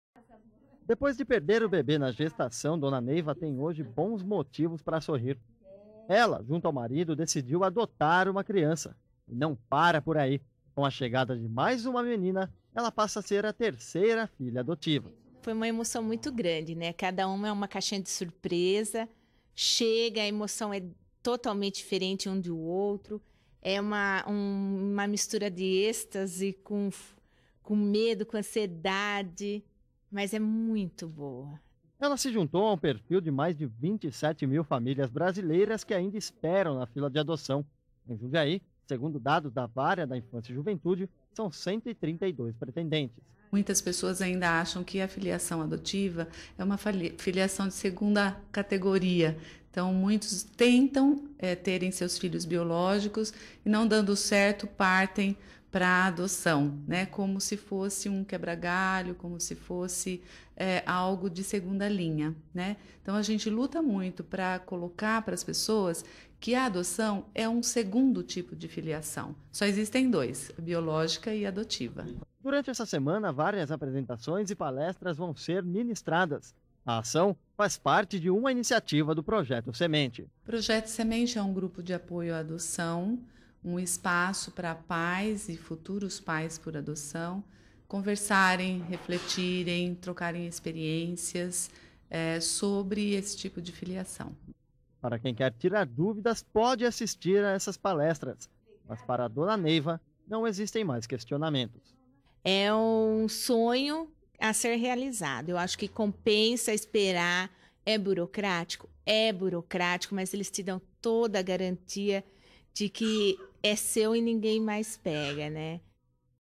Semana da Adoção
Reportagem realizada pela TV Educativa de Jundiaí. (05-12)